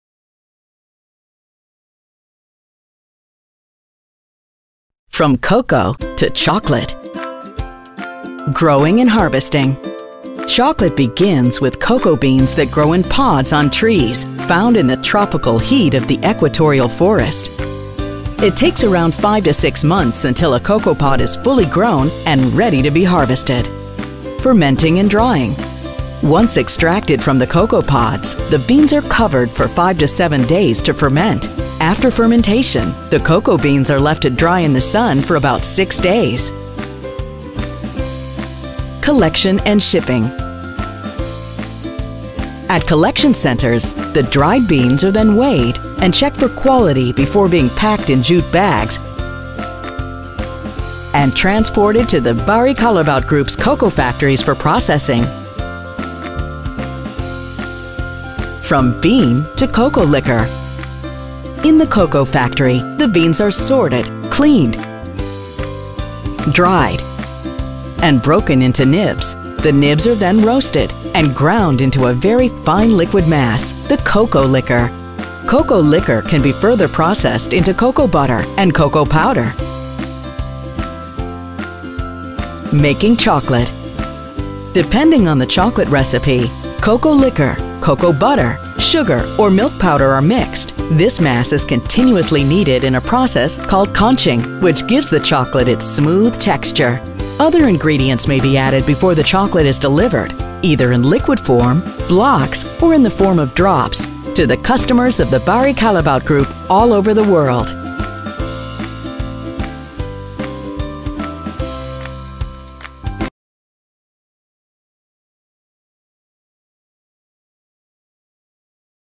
Dự án Animation thực tế Red Cat Academy: Quy trình chế biến Cacao thành Chocolate.
Các bạn có biết đâu là nguồn gốc tạo nên những thanh Chocolate ngon lành không? Đó là cả một quy trình phức tạp từ việc thu hoạch quả Cacao ngon lành cho đến dây chuyền xử lý và chế biến các nguyên liệu, nhưng đã được các học viên Red Cat Academy truyền tải một cách thú vị và dễ hiểu qua dự án Animation quy trình chế biến Cacao thành Chocolate

Lớp Tư Duy Sáng Tạo và Motion Cơ Bản R010
Giảng viên: Minh Nguyễn, Leo Dinh
Học viên: Nguyễn Tất Viễn, Hắc Tâm, Nguyễn Ngọc, Đoàn Sĩ Phú.

Cùng tìm hiểu quy trình làm việc của cả nhóm tại đây nhé